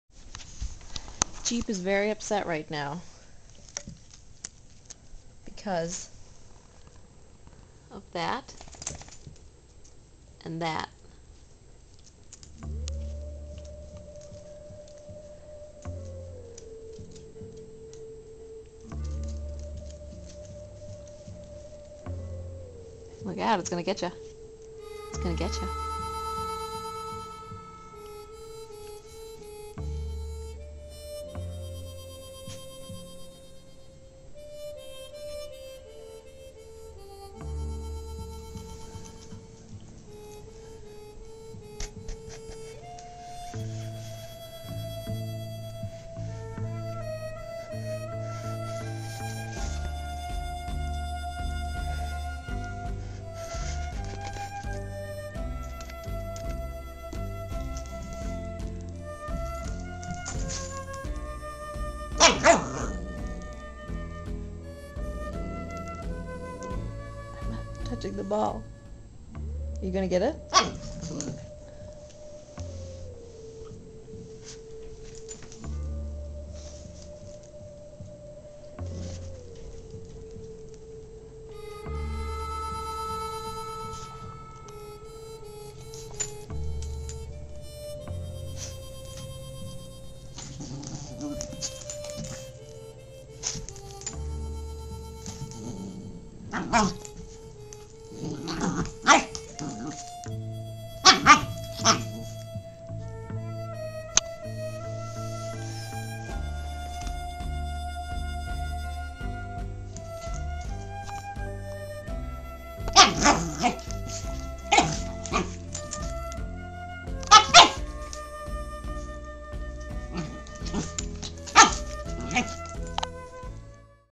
Wire Fox Terrier Puppy in a Standoff
Jeep (here about four months old) tries to stand up to a beach ball.

(She is in between her puppy coat and her adult coat, which is why she looks a little rough around the edges.)